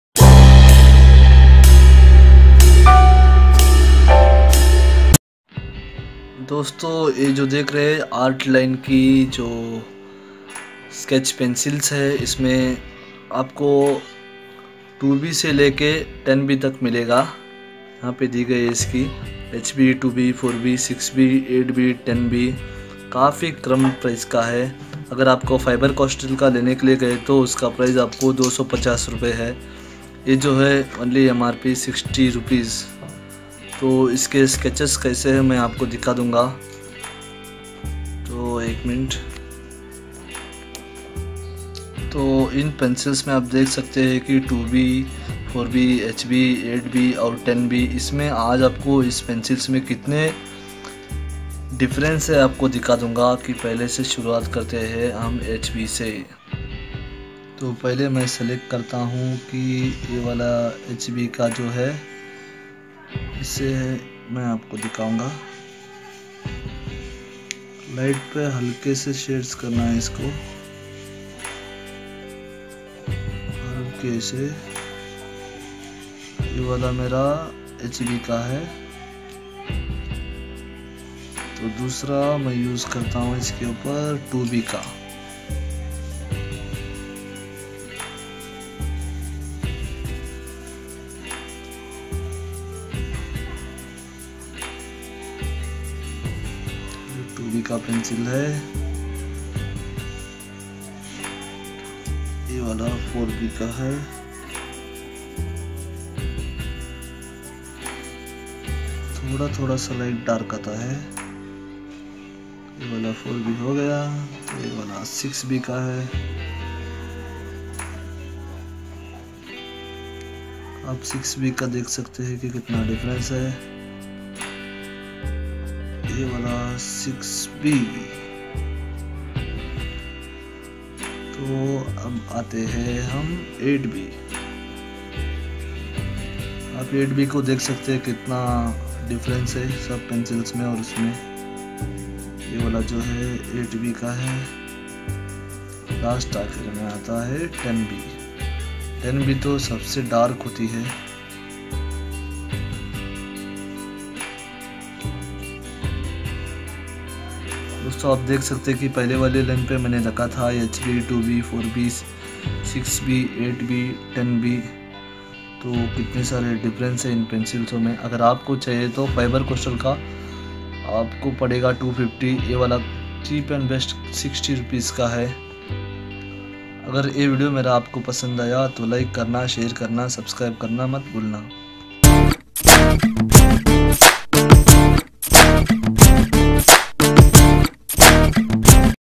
chip and best sketching pencils you see defrent shades
hi freind today i gon show you chip and best sketching pencils this pencil brand was artline its very chip pencils and good colity